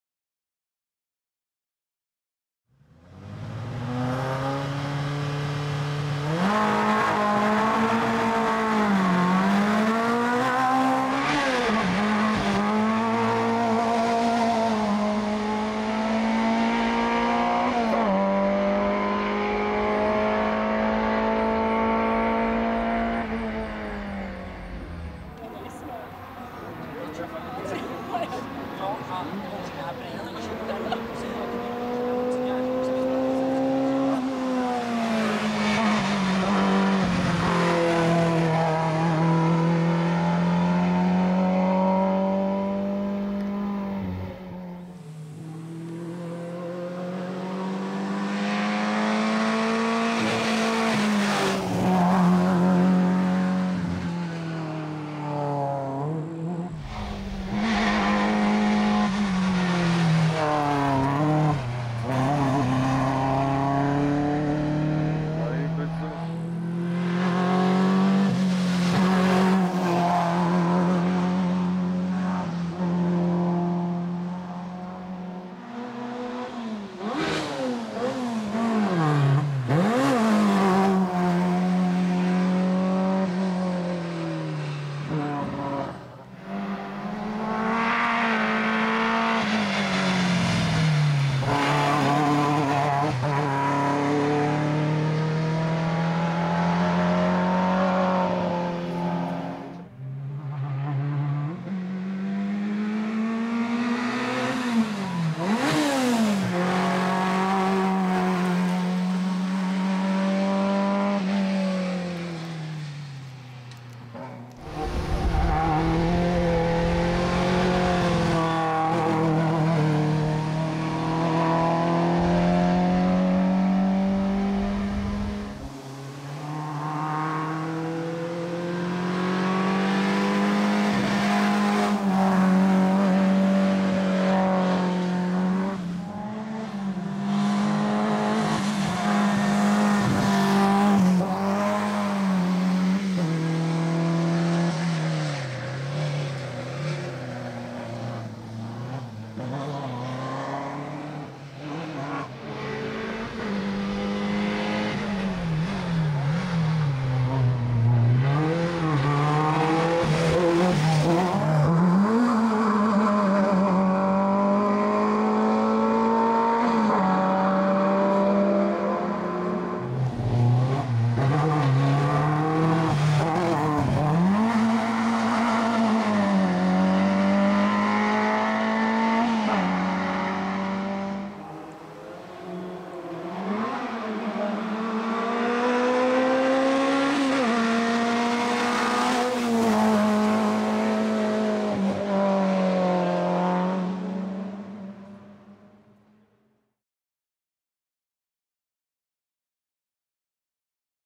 Škoda Felicia Kit Car - Vladimír Žid - Bozkov-Cimbál 2024 - EDDA CUP
Vladimír Žid si s vozem Škoda Felicia Kit Car dojel v sobotu pro osmé místo ve třídě E-1400 v závodech do vrchu Bozkov-Cimbál 2024 v seriálu EDDA CUP.